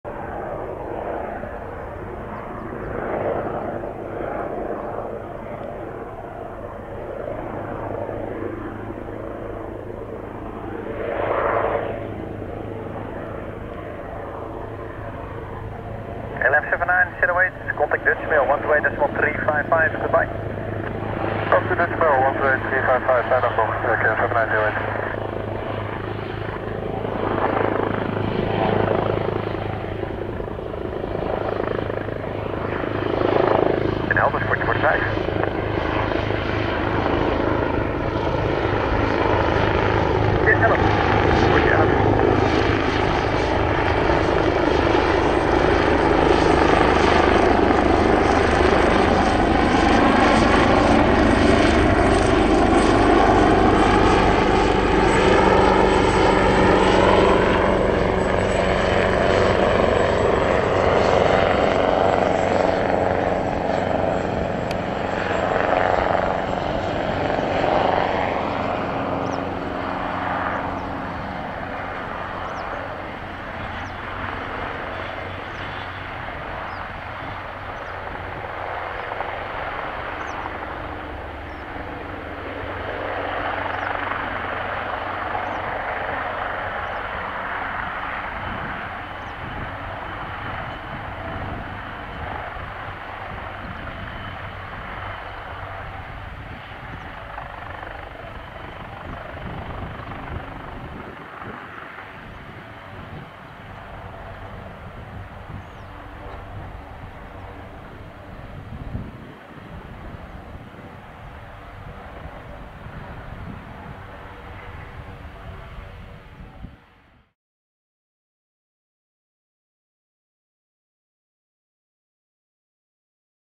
[4K] CHC Helicopters AW-139 PH-EUK coming in after offshore flight | DHR/EHKD
April 14, 2022: Landing of PH-EUK at Den Helder Airport coming in from an offshore mission.